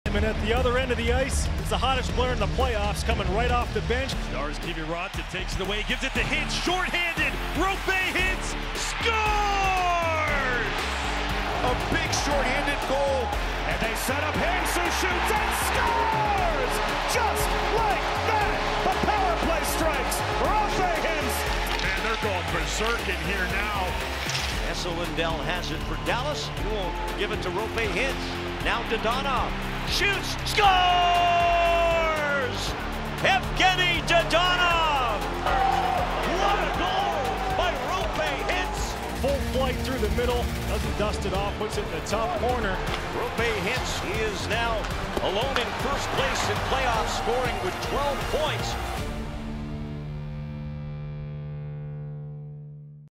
🇫🇮 Roope Hintziltä hulppea suoritus 1. kierroksella Wildiä vastaan
Roope Hintz teki viisi maalia ja antoi seitsemän syöttöä, kun Stars löi Wildin voitoin 4-2 pudotuspelien 1. Kierroksella.

Feeling social?